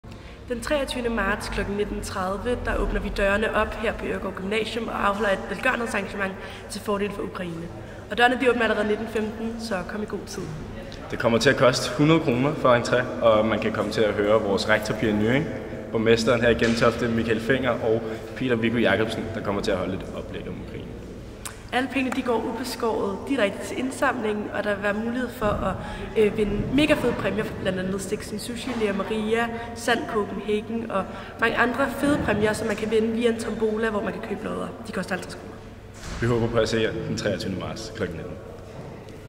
Vi støtter Ukraine 🇺🇦 | Øregård Gymnasium
Onsdag den 23. marts kl. 19.30 afholder Øregård Gymnasium i samarbejde med Dansk Røde Kors et velgørenhedsarrangement til fordel for Dansk Røde Kors' humanitære indsats i Ukraine. Arrangementet er både for elever, deres familier og interesserede fra lokalsamfundet.
🇺🇦 Peter Viggo Jakobsen, som har en ph.d. i international politik, er lektor på Forsvarsakademiet og ekspertkilde i talrige nyhedsudsendelser, kommer og holder oplæg om situationen. Der vil i den forbindelse kunne stilles spørgsmål til krigen og dens konsekvenser.
🇺🇦 Derudover vil Gentoftes borgmester, Michael Fenger, og rektor på Øregård Gymnasium, Pia Nyring begge holde oplæg om situationen.
🇺🇦 Lokale forretninger har doneret gavmildt til arrangementet, så der er mulighed for at vinde spændende og eksklusive præmier fra Ole Lynggaard, Leah Maria, SAND Copenhagen og mange andre vilde præmier.
🇺🇦 Arrangementet koster kr. 100,- pengene går ubeskåret til Dansk Røde Kors’ støttearbejde. Billetter købes i indgangen.
🇺🇦 Del meget gerne opslaget, så vi kan få skabt awareness om arrangementet. Alle er velkomne.